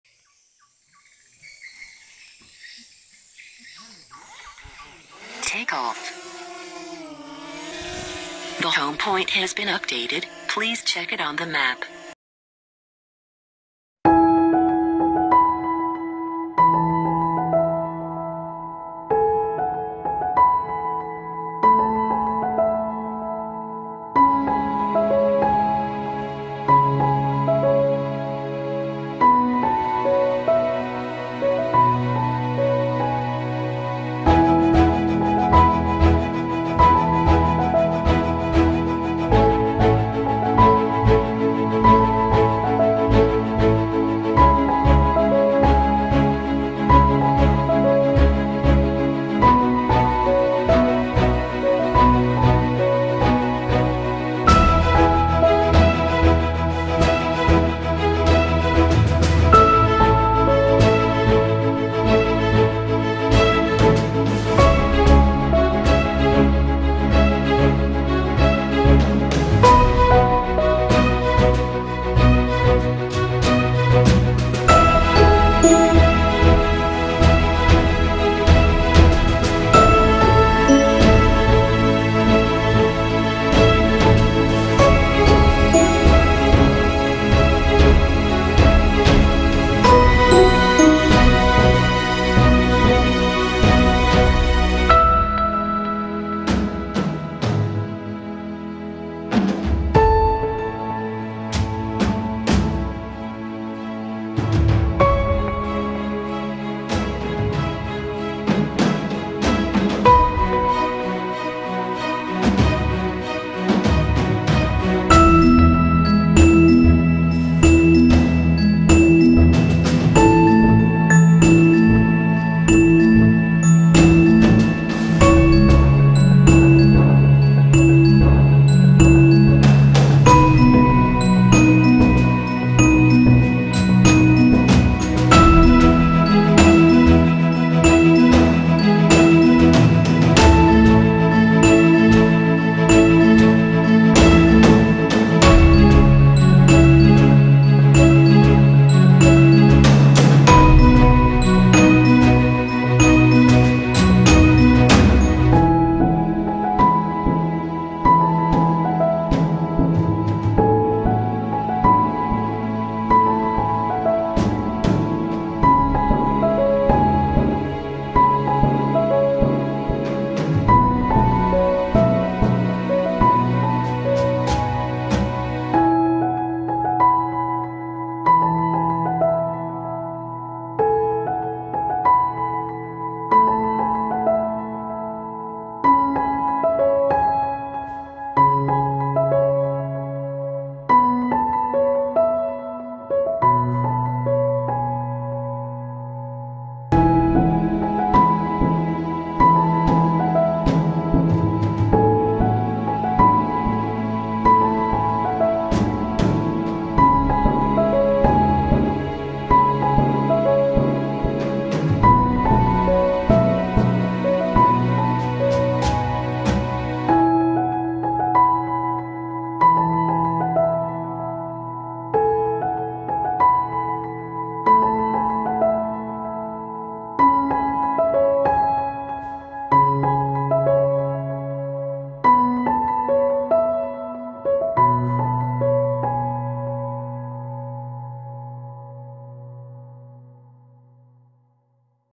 DJI mavic mini Kampung Wong Pandak Lubok Antu Sarawak (HD available)
High Definition available.
Aerial view of Kampung Wong Pandak Lubok Antu.
Located in Sri Aman Sarawak. Nearby Batang Ai Dam.
Video Editor: VSDC free edition